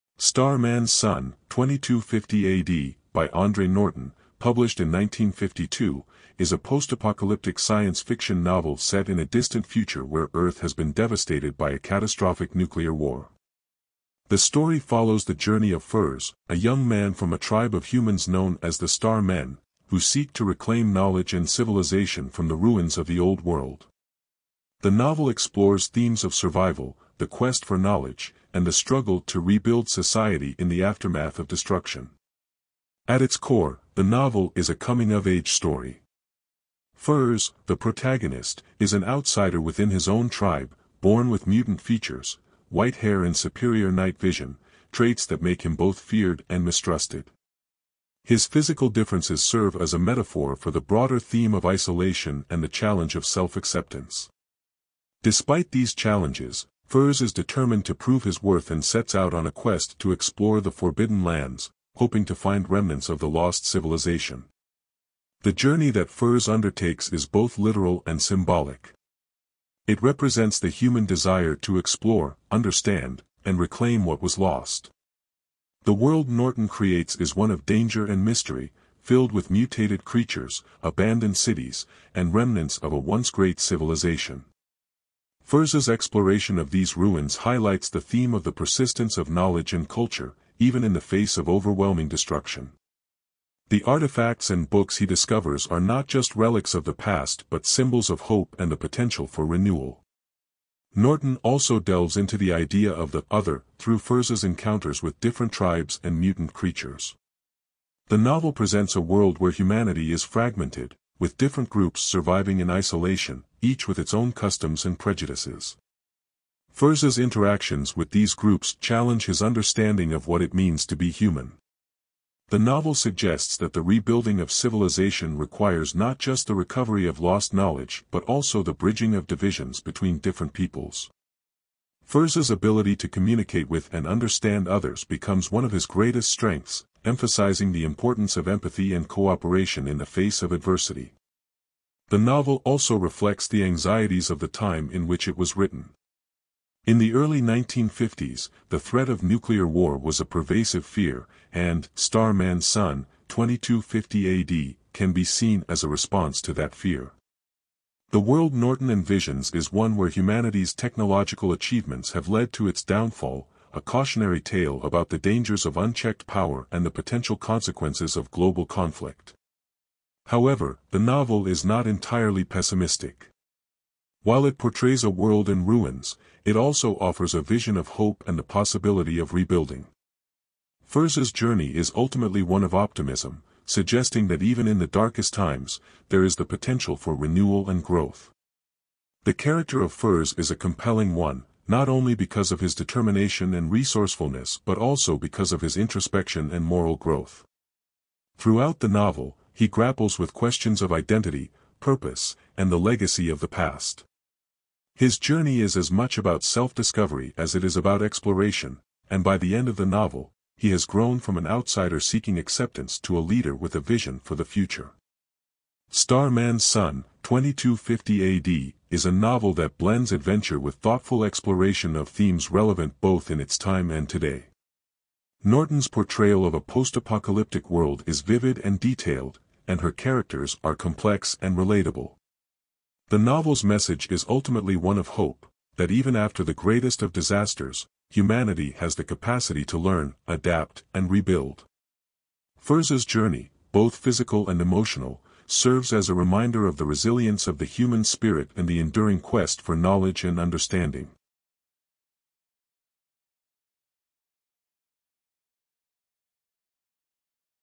"Star Man's Son, 2250 A.D" By Andre Norton
"Star Man's Son, 2250 A.D." by Andre Norton, published in 1952, is a post-apocalyptic science fiction novel set in a distant future where Earth has been devastated by a catastrophic nuclear war. The story follows the journey of Fors, a young man from a tribe of humans known as the Star Men, who seek to reclaim knowledge and civilization from the ruins of the old world. The novel explores themes of survival, the quest for knowledge, and the struggle to rebuild society in the aftermath of destruction.At its core, the novel is a coming-of-age story. Fors, the protagonist, is an outsider within his own tribe, born with mutant features—white hair and superior night vision—traits that make him both feared and mistrusted. His physical differences serve as a metaphor for the broader theme of isolation and the challenge of self-acceptance. Despite these challenges, Fors is determined to prove his worth and sets out on a quest to explore the forbidden lands, hoping to find remnants of the lost civilization.The journey that Fors undertakes is both literal and symbolic. It represents the human desire to explore, understand, and reclaim what was lost. The world Norton creates is one of danger and mystery, filled with mutated creatures, abandoned cities, and remnants of a once-great civilization. Fors's exploration of these ruins highlights the theme of the persistence of knowledge and culture, even in the face of overwhelming destruction. The artifacts and books he discovers are not just relics of the past but symbols of hope and the potential for renewal.Norton also delves into the idea of the "Other" through Fors's encounters with different tribes and mutant creatures. The novel presents a world where humanity is fragmented, with different groups surviving in isolation, each with its own customs and prejudices. Fors's interactions with these groups challenge his understanding of what it means to be human. The novel suggests that the rebuilding of civilization requires not just the recovery of lost knowledge but also the bridging of divisions between different peoples. Fors's ability to communicate with and understand others becomes one of his greatest strengths, emphasizing the importance of empathy and cooperation in the face of adversity.The novel also reflects the anxieties of the time in which it was written. In the early 1950s, the threat of nuclear war was a pervasive fear, and "Star Man's Son, 2250 A.D." can be seen as a response to that fear. The world Norton envisions is one where humanity's technological achievements have led to its downfall, a cautionary tale about the dangers of unchecked power and the potential consequences of global conflict. However, the novel is not entirely pessimistic. While it portrays a world in ruins, it also offers a vision of hope and the possibility of rebuilding. Fors's journey is ultimately one of optimism, suggesting that even in the darkest times, there is the potential for renewal and growth.The character of Fors is a compelling one, not only because of his determination and resourcefulness but also because of his introspection and moral growth. Throughout the novel, he grapples with questions of identity, purpose, and the legacy of the past. His journey is as much about self-discovery as it is about exploration, and by the end of the novel, he has grown from an outsider seeking acceptance to a leader with a vision for the future."Star Man's Son, 2250 A.D." is a novel that blends adventure with thoughtful exploration of themes relevant both in its time and today. Norton's portrayal of a post-apocalyptic world is vivid and detailed, and her characters are complex and relatable. The novel's message is ultimately one of hope: that even after the greatest of disasters, humanity has the capacity to learn, adapt, and rebuild. Fors's journey, both physical and emotional, serves as a reminder of the resilience of the human spirit and the enduring quest for knowledge and understanding.